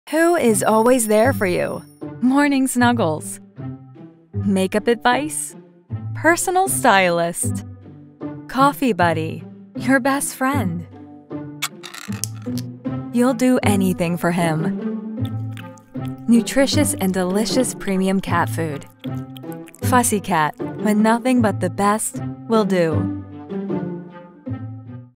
Who is always there for you? Presented by Fussie Cat
Morning  snuggles
Make-up advice
Personal stylist
Coffee buddy
Your best friend
You'll do anything for him
Nutritious and delicious premium cat food.
Fussie Cat: When nothing but the best will do.